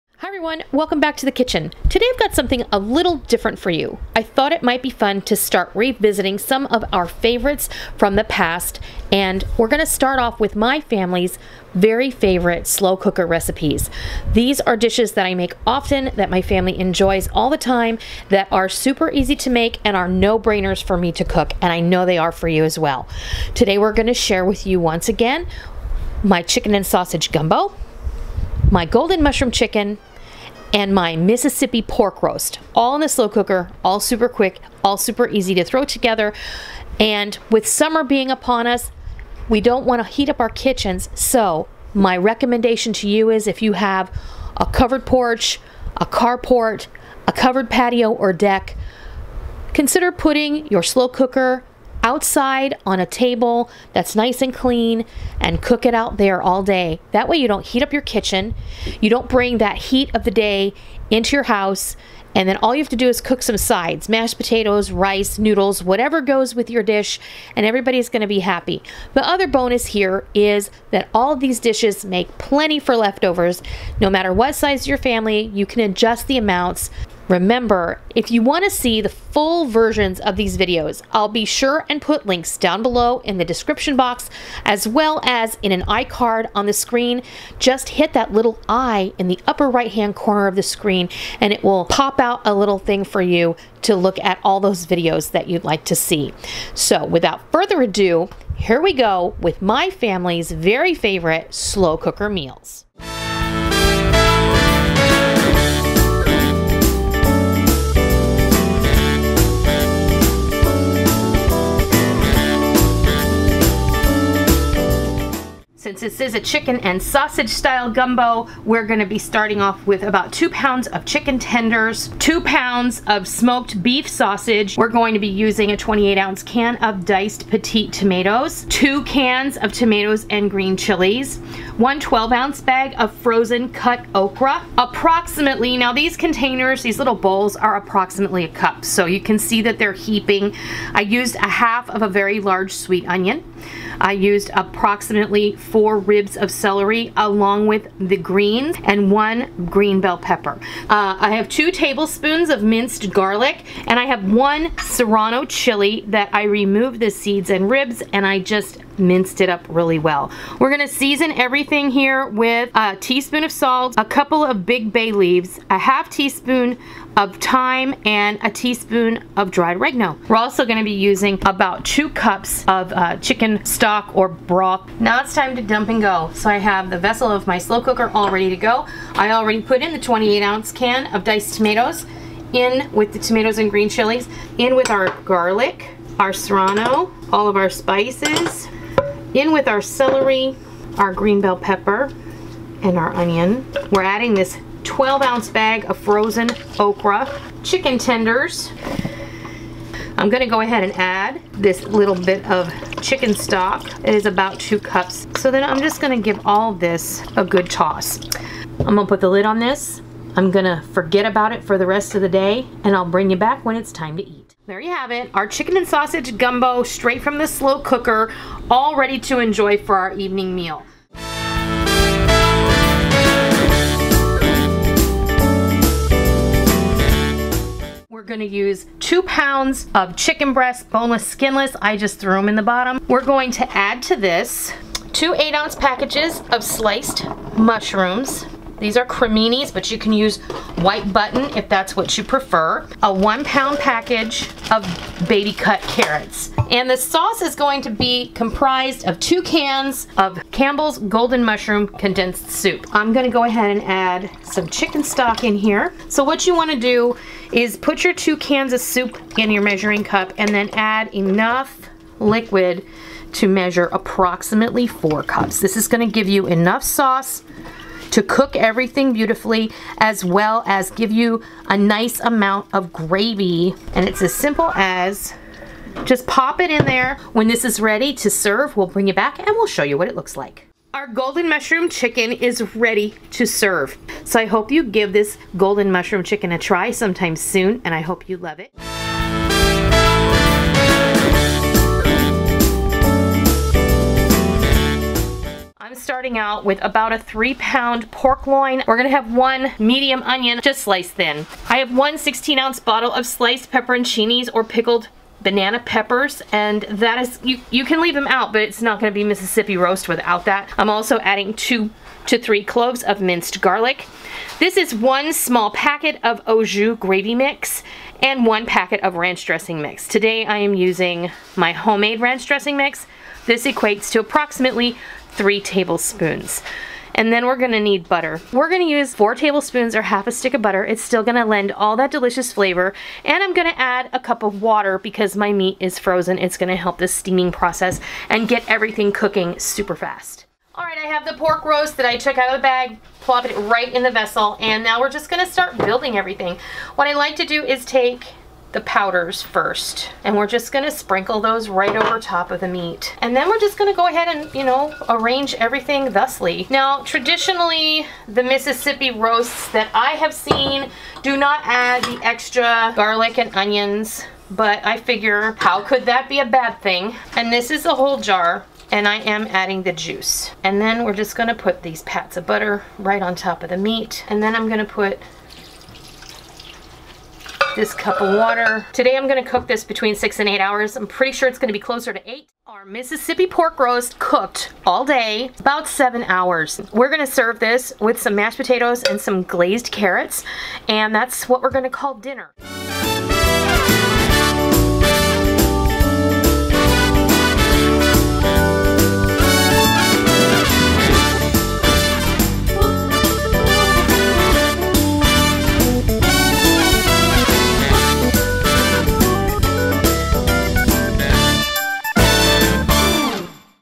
3 FAMILY FAVORITE SLOW COOKER DINNER IDEAS!!
Links to all the full version videos as well as recipes here!

Trying something a bit different today!  Going to start sharing some videos where we revisit family favorites of certain categories!  It seems that some people really enjoy these type of videos!  These help remind you of good ideas and for those who never saw them, it gives them a quick taste of what they can see in the full version!  I hope you enjoy this video featuring three of my family's favorite slow cooker dinners that are my "go to" meals on days when cooking is just going to need to be done without thinking!

***MAILING ADDRESS
Noreen's Kitchen